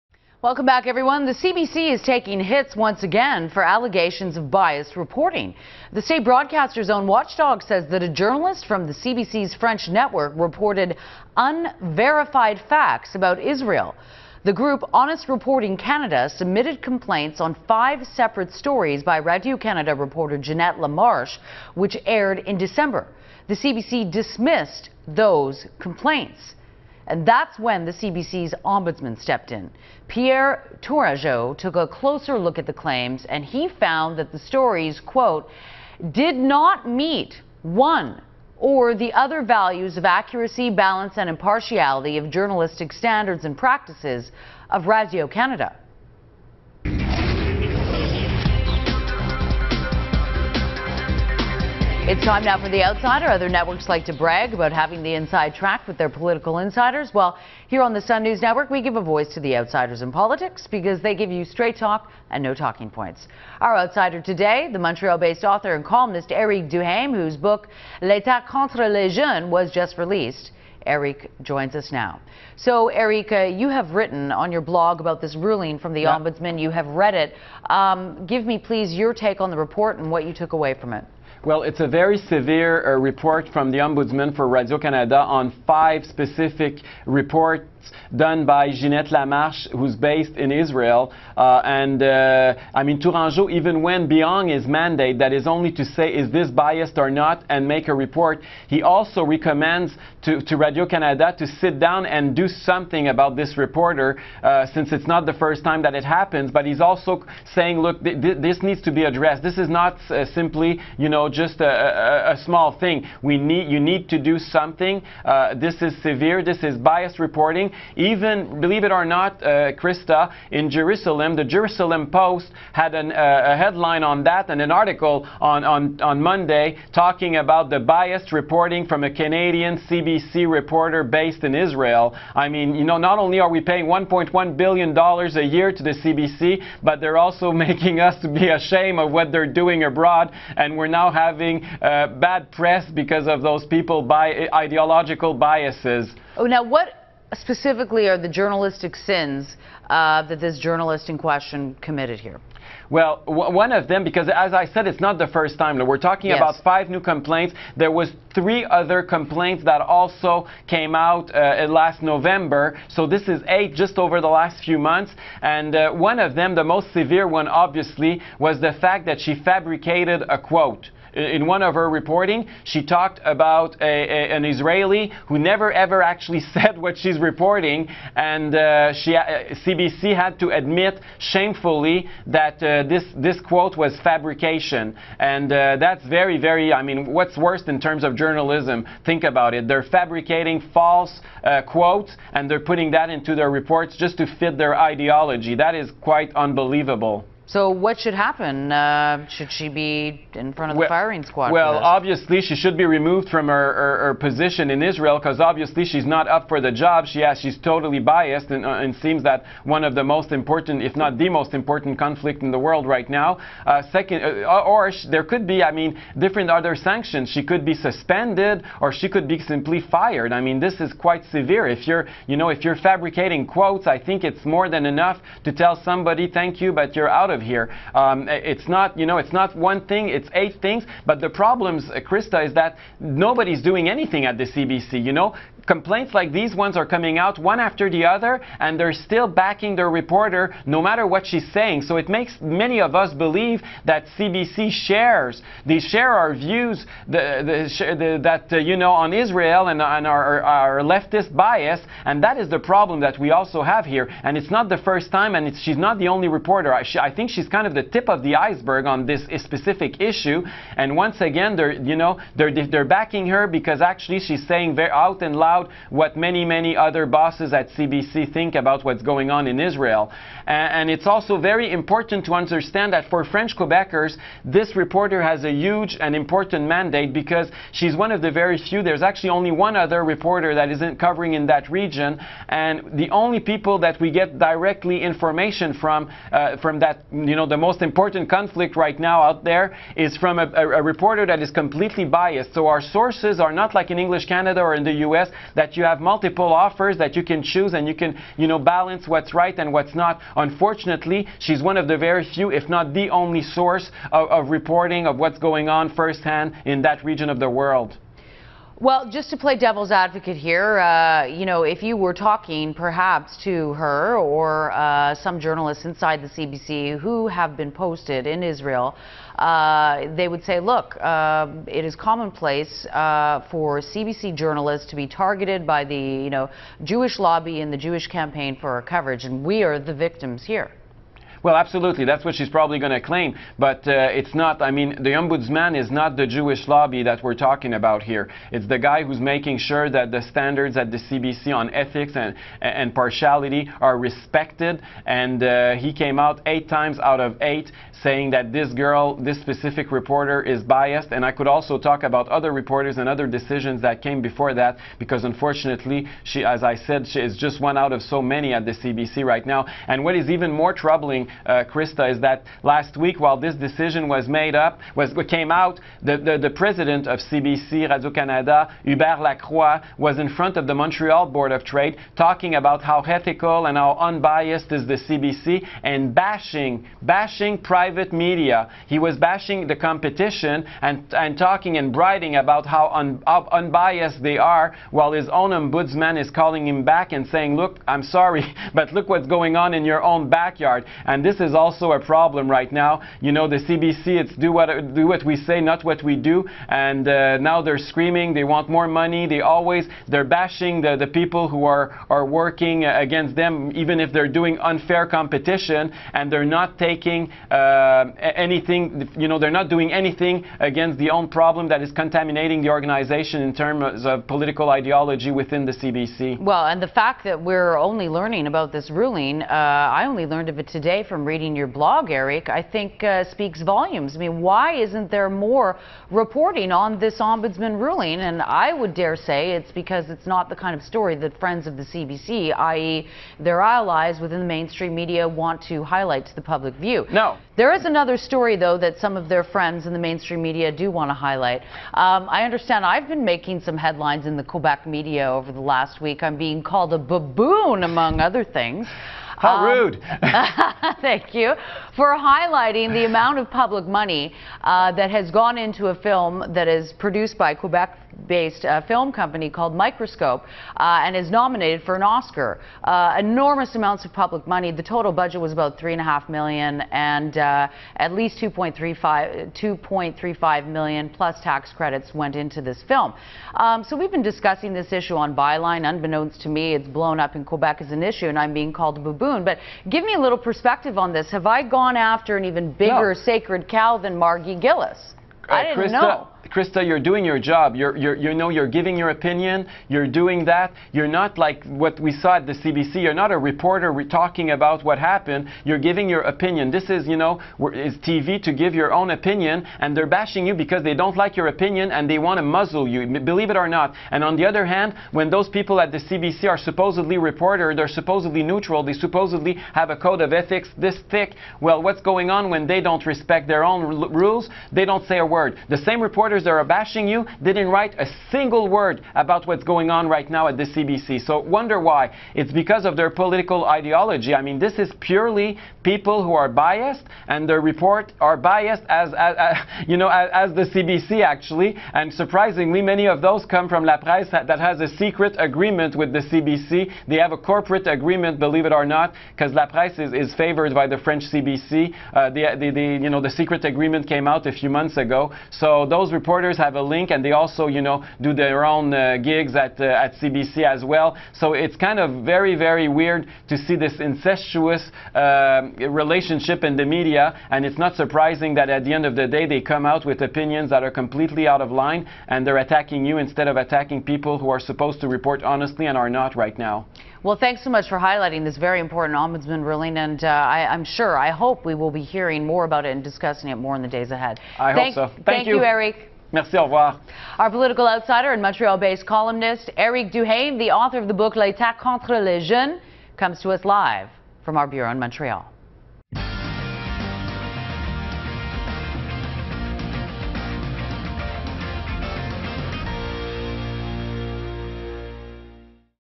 CBC ombudsman slams CBC
The CBC's own watchdog has found reporting on Israel by the state broadcaster did not meet values of accuracy, balance, and impartiality. Eric Duhaime joined Krista Erickson to discuss.